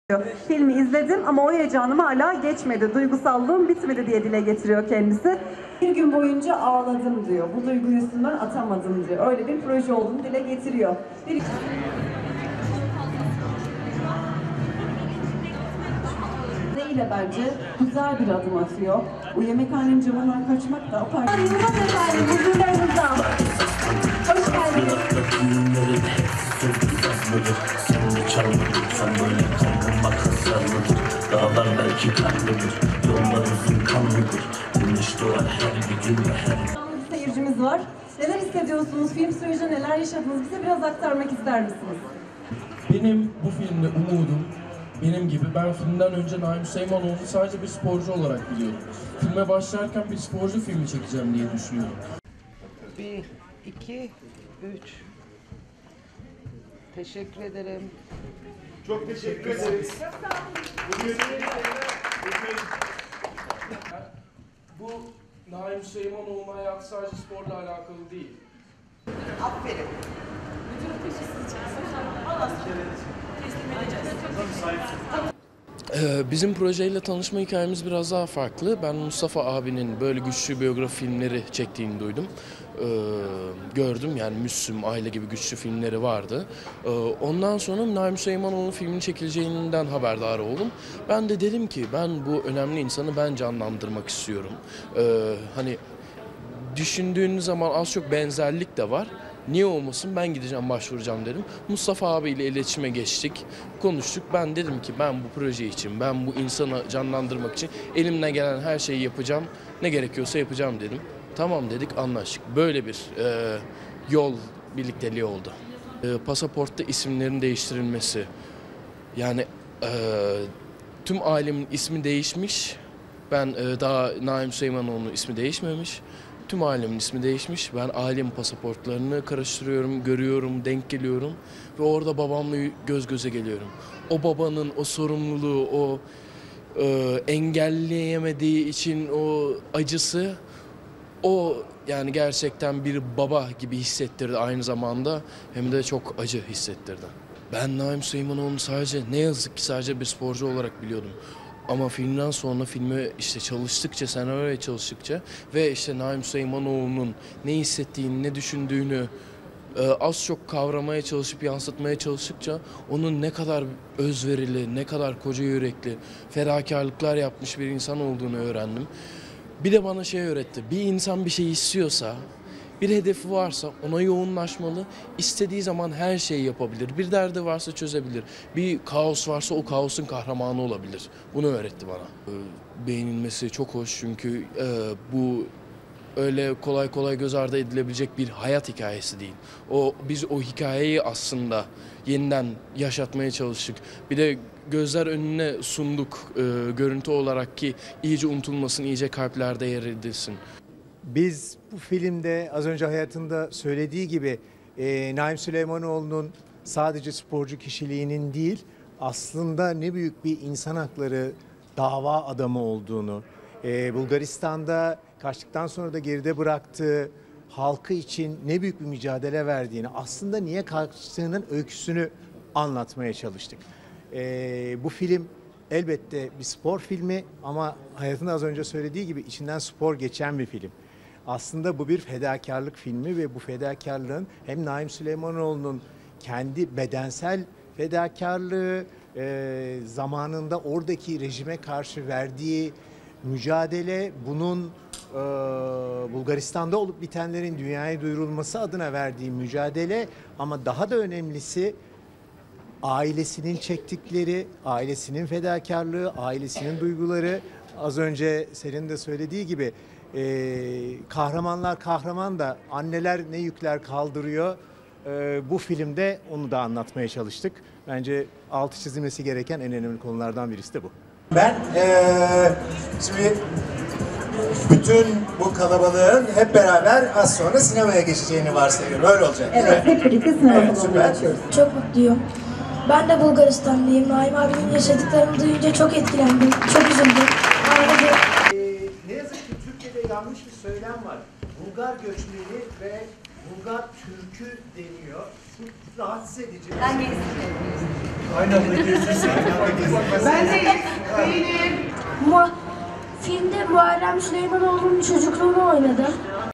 'Naim Süleymanoğlu' filmi oyuncuları Eskişehirlilerle buluştu
“Cep Herkülü: Naim Süleymanoğlu” filminin oyuncuları Eskişehir’de sinemaseverler ile buluştu.